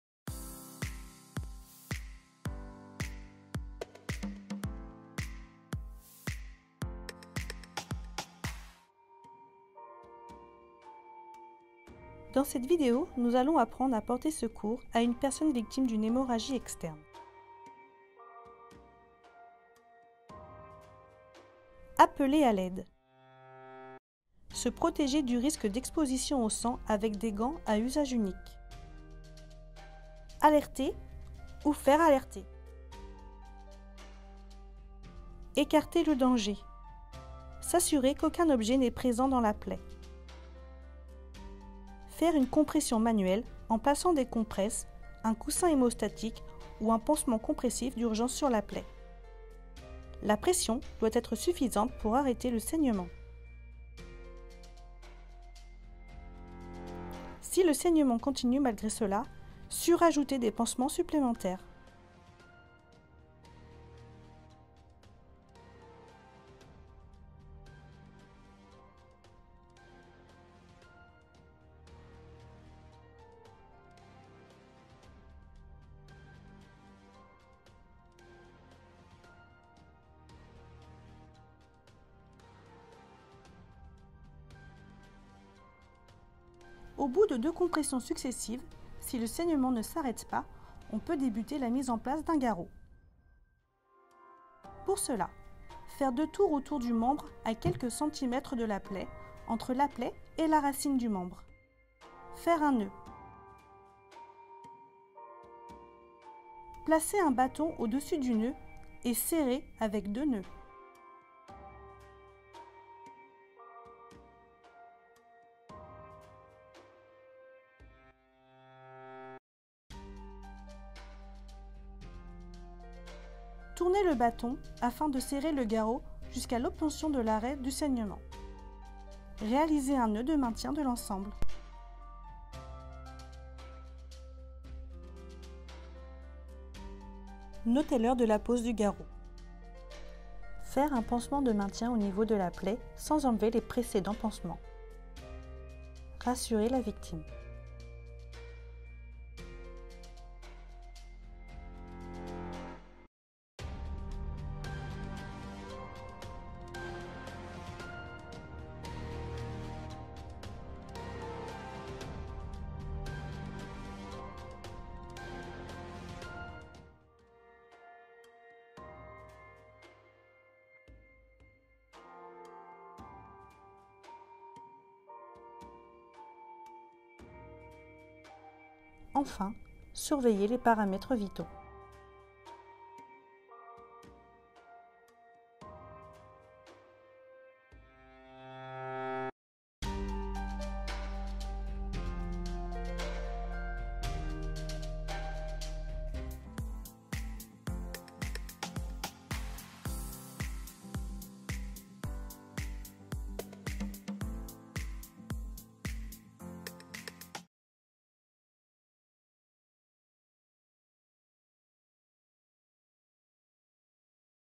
Prise en charge d’un patient victime d’hémorragie
Une hémorragie externe est un épanchement de sang abondant et visible, qui s’écoule en dehors des vaisseaux au travers d’une plaie et ne s’arrête pas spontanément. L’hémorragie externe est le plus souvent d’origine traumatique (coup, chute, couteau, balle...), plus rarement médicale (rupture de varices).

L’action de secours doit permettre :
- d’arrêter immédiatement l’hémorragie pour limiter la perte de sang ;
- de prendre les mesures nécessaires pour limiter ses conséquences.